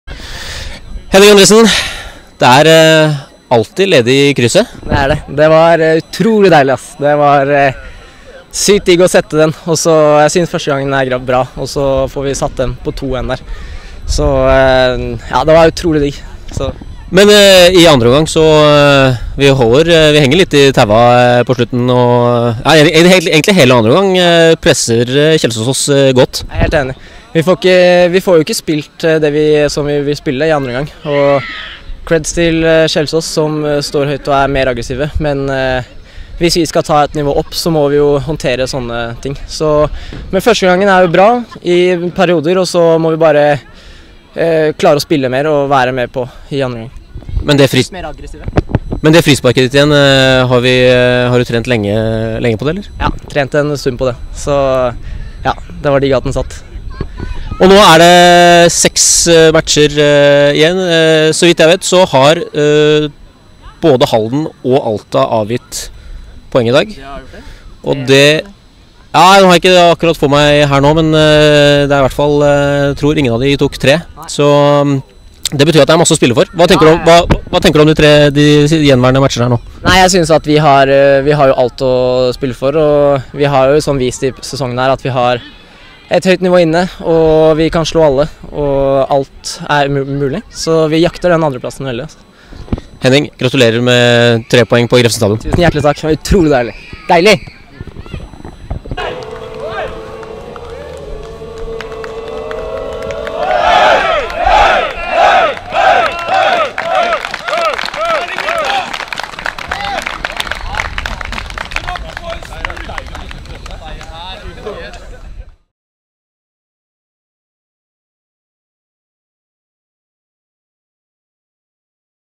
Intervju med Henning Andresen etter Kjelsås - Skeid 03.10.2020
Skeid vant 3-2.